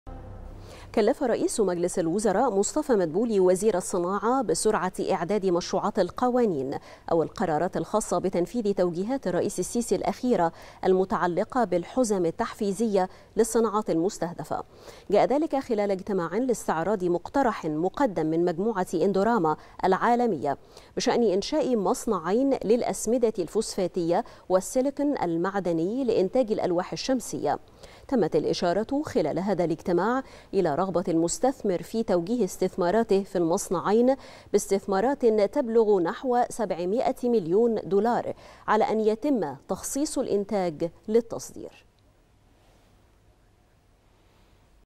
مدبولي يكلف بسرعة إعداد مشروعات القوانين والقرارات الخاصة بتنفيذ توجيهات الرئيس السيسي
نشرة الثالثة | مدبولي يكلف بسرعة إعداد مشروعات القوانين والقرارات الخاصة بتنفيذ توجيهات الرئيس السيسي المتعلقة بتحفيز الصناعات المستهدفة